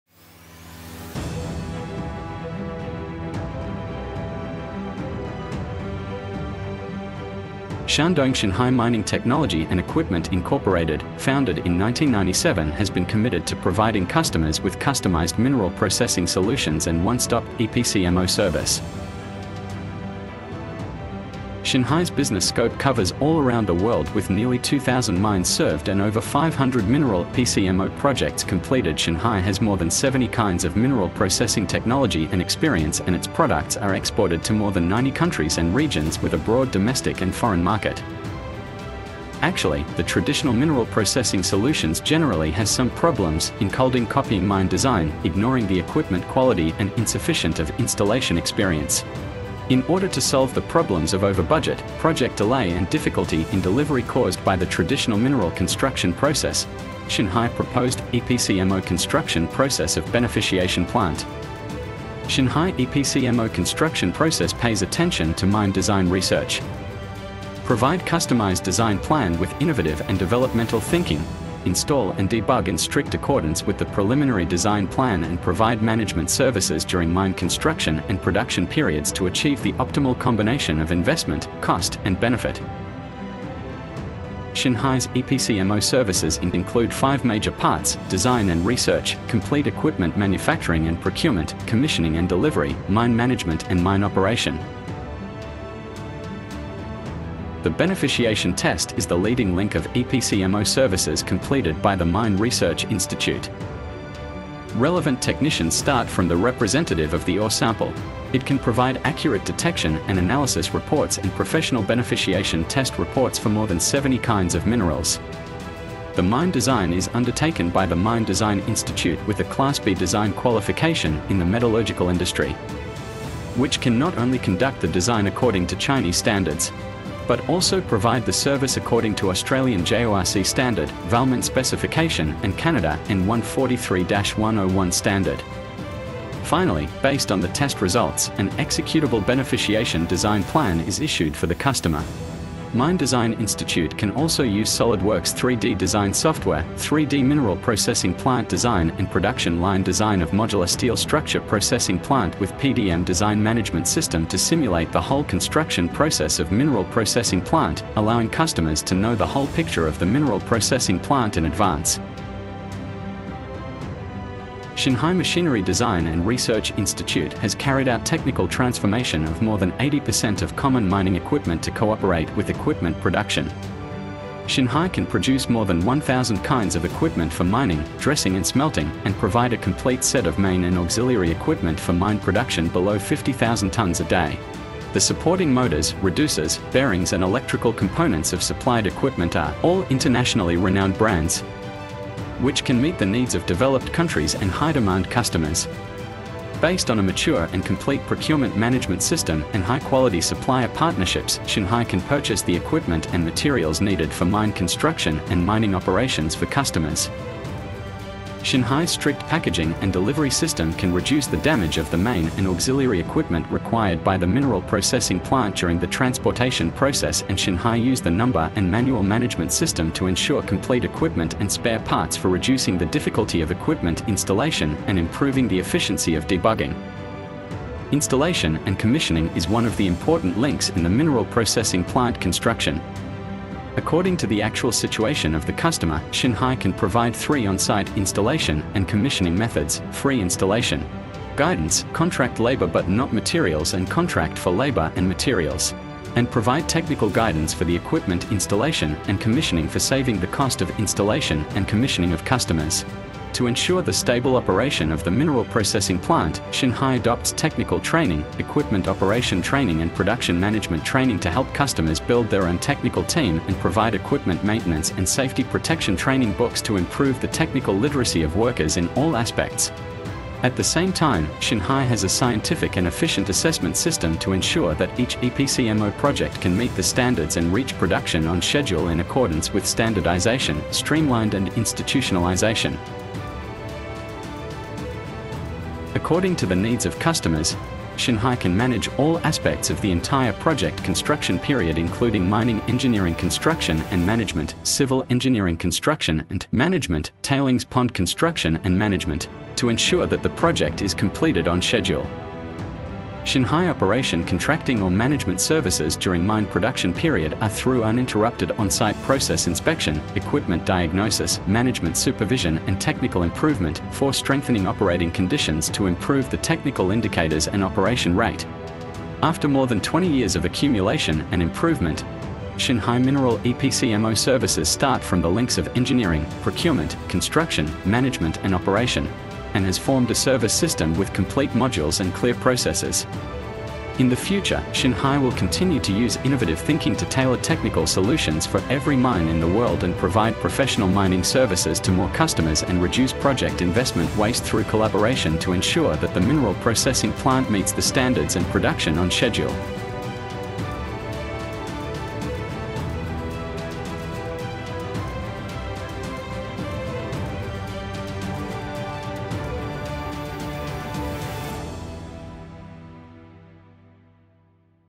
Xinhai Mineral Processing EPC+M+O Service|2023
Optimized Solution for Your Mine!
Xinhai Mining Technology & Equipment Inc. stands at the forefront of modern mineral processing solutions through its comprehensive EPC+M+O service model. As the pioneer and practitioner of the "Turn-key Solution for Mineral Processing Plant" concept, our company integrates cutting-edge technologies with operational excellence to deliver complete project life cycle services.

Our EPC+M+O service represents the core competency that distinguishes Xinhai in the global mining sector. This integrated approach encompasses the entire value chain from initial design to ongoing operations, specifically including: Engineering design and research, Complete equipment manufacturing and procurement, Commissioning and delivery, Mine management, and Mine operation services. The M+O component is meticulously tailored to each project's specific operational requirements.

With an extensive track record spanning over 2,000 mine projects worldwide, Xinhai has successfully executed 500 complete EPC+M+O projects. Our technological expertise covers 70 different ore types, supported by numerous patented innovations. This substantial experience is complemented by our global presence, with overseas offices strategically located across multiple countries and our equipment serving clients in over 90 nations.

TURN-KEY SOLUTIONS FOR MINERAL PROCESSING PLANT (EPC+M+O)
ENGINEERING SERVICES
Our engineering capabilities include comprehensive consultant services, mineral processing testing, process flow development, optimal equipment selection, complete plant design, and detailed construction drawings. Each project benefits from our rigorous technical analysis and innovative engineering solutions developed through decades of practical experience.

PROCUREMENT SOLUTIONS
Xinhai's procurement services encompass the complete spectrum of mining requirements: specialized mineral processing equipment, mine construction materials, installation tools, maintenance workshop devices, laboratory setups, modular building production lines, and structural components. We also provide specialized chemical solutions including environmentally-friendly gold leaching reagents as alternatives to sodium cyanide, along with various flotation reagents.

CONSTRUCTION MANAGEMENT
Our construction services feature expert guidance for plant construction and equipment installation, comprehensive commissioning procedures, and thorough staff training programs. We ensure operational readiness through spare parts provisioning, consumables supply, equipment maintenance systems, and specialized services including gold smelting operations.

MANAGEMENT AND OPERATION SERVICES
The M+O component represents Xinhai's commitment to long-term project success, offering both construction period management and production phase operation services. These include specialized mining engineering support, civil engineering oversight, tailings pond construction management, and complete daily operational management solutions.

"Excellence and Meticulosity" remains the fundamental principle guiding every aspect of our service delivery. This philosophy ensures that each project phase receives meticulous attention, from initial design through to continuous operations. Our EPC+M+O model ultimately delivers superior value through integrated solutions that optimize efficiency, reduce operational risks, and maximize resource recovery throughout the mine life cycle.